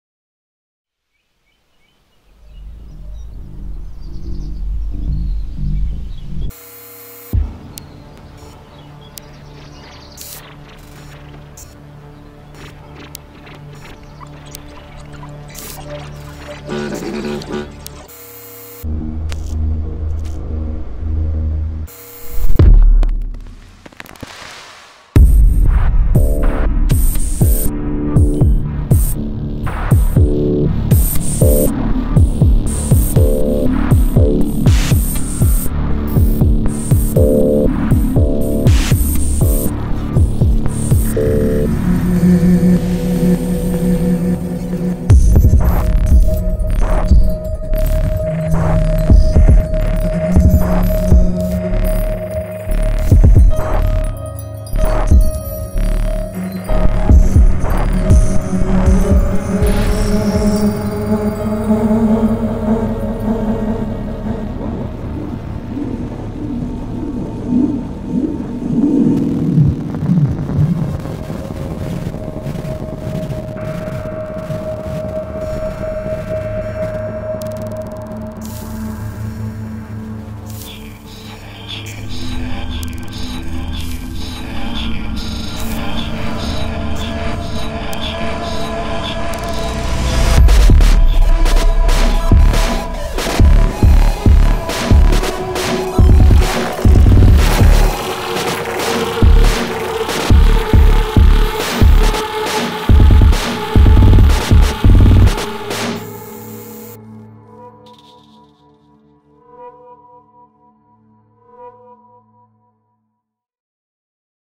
Syndrome - Max/msp Instruments in Live Installation
Using max msp programming we built instrument for live shows and music creation: looping, sampling, granular synthesizing and voice effects.